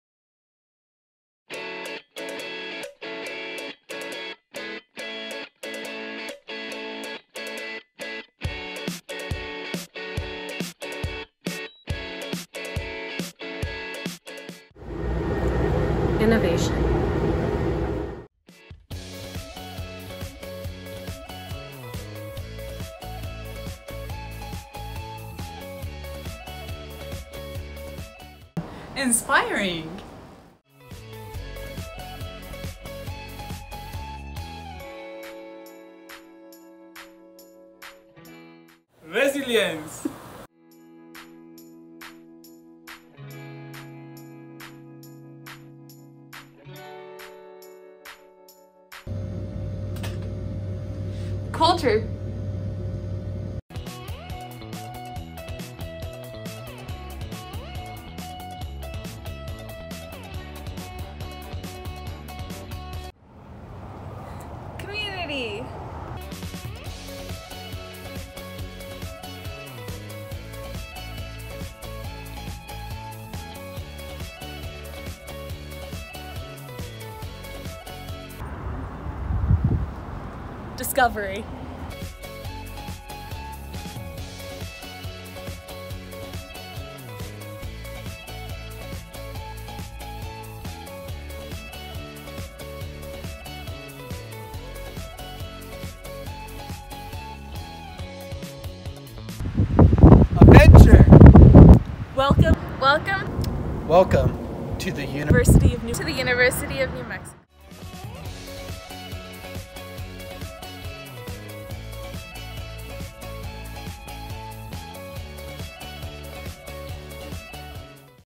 UNM Biomedical Sciences Graduate Program 2020
Students and faculty in The University of New Mexico Biomedical Sciences Program explain why UNM is a great place to live and learn.